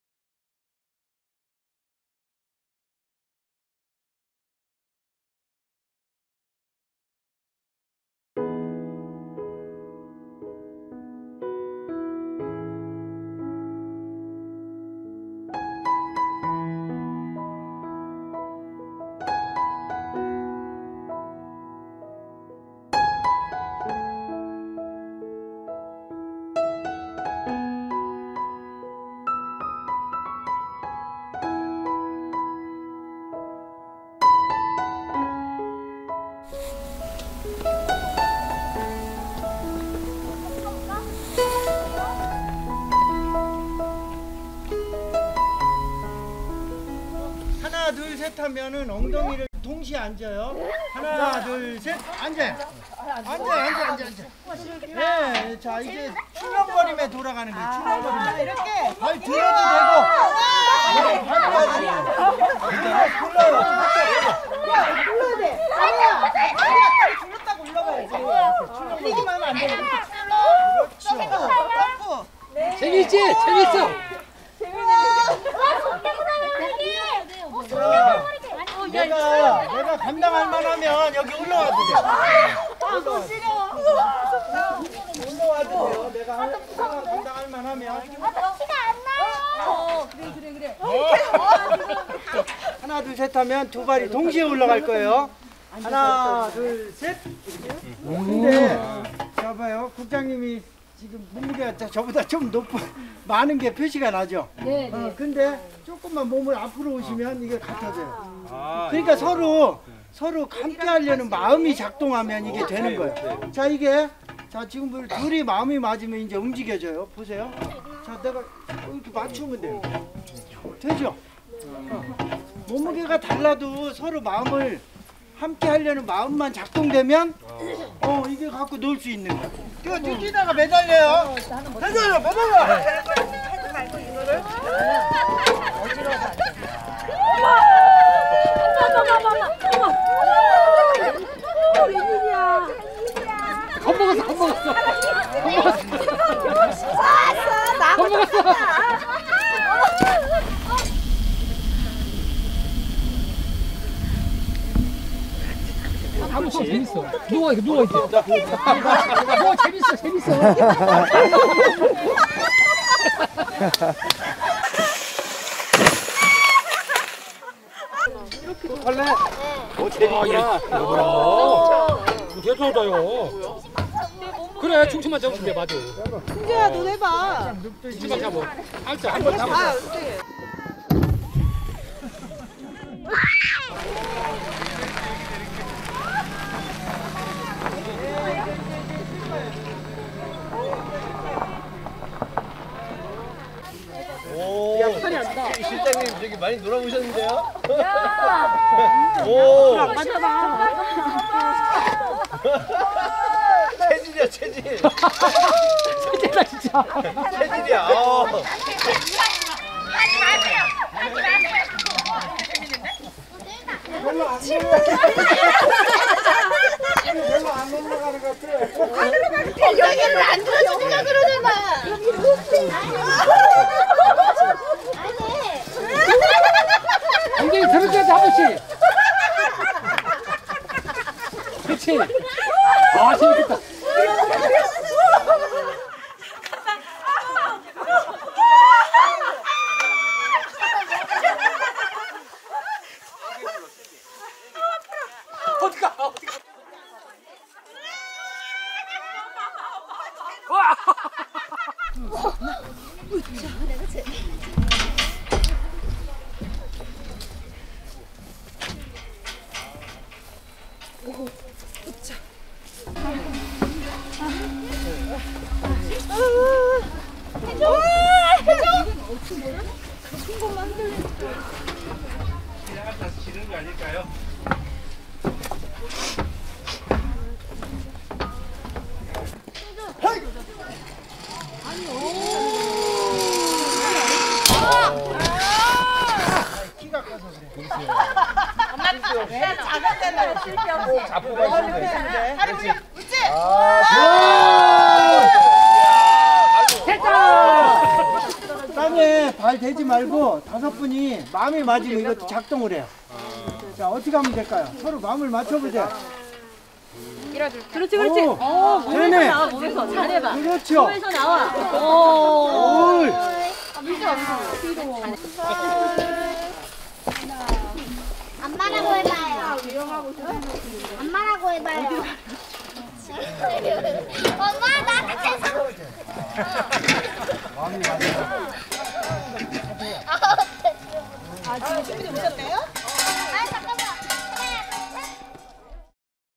비전력놀이공원체험
2023년 2월 18일부터 19일까지
옥천 아자학교에서 열린 환경콘서트
1일차 첫번째 행사인 한국최초 비전력놀이공원 체험 영상